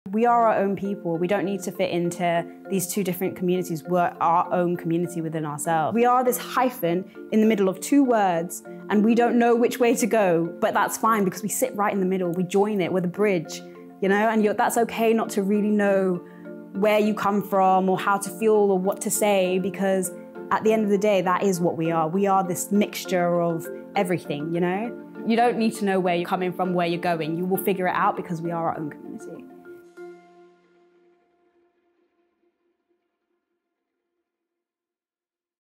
Indo-Caribbean Londoners | ‘We are our own people’
Indo-Caribbean Londoners share their stories – Jana Ally

Display closes 19th November 2023